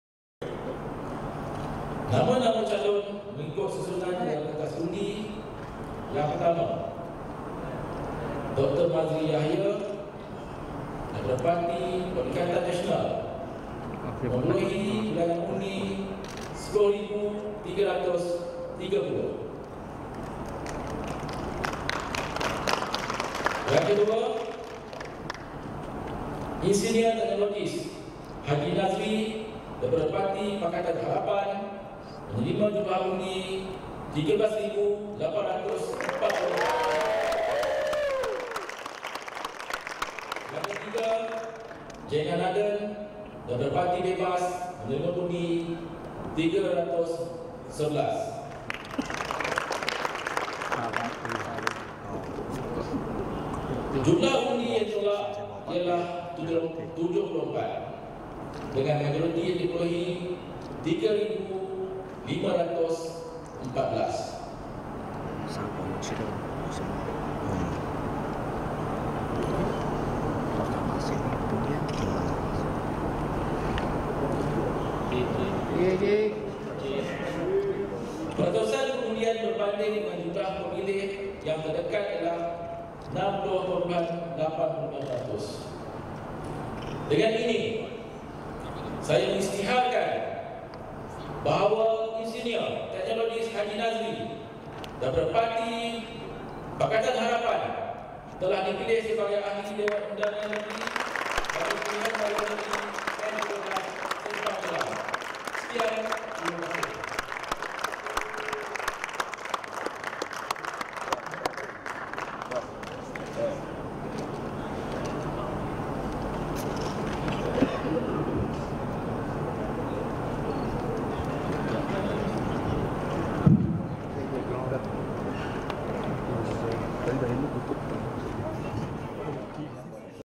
PH menang di DUN Simpang Jeram, jurutera pilihan pengundi
Pakatan Harapan (PH) berjaya meneruskan tampuk pimpinan di Dewan Undangan Negeri (DUN) Simpang Jeram, sekali gus melenyapkan stigma gelombang Perikatan Nasional (PN) di kawasan itu.
Calon PH, Nazri Abdul Rahman yang juga seorang jurutera menang dengan majoriti 3,514 selepas meraih 13,844 undi menewaskan pakar bedah, Dr Mohd Mazri Yahya yang meraih 10,330 undi manakala calon Bebas S.Jeganathan hanya mendapat 331 undi.